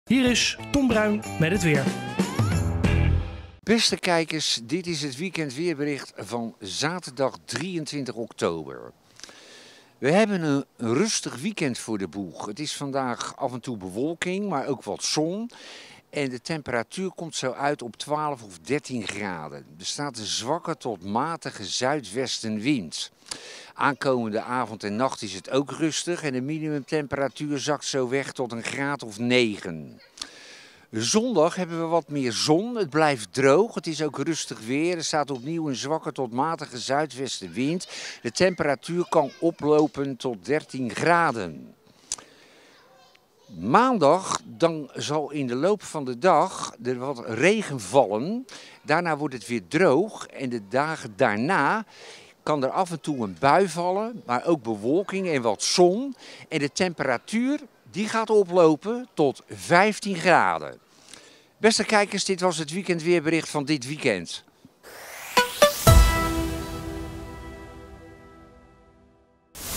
SCHIE TV: Het weekendweer met Tom Bruin
Weerman Tom Bruin vertelt wat voor weer we dit weekend kunnen verwachten.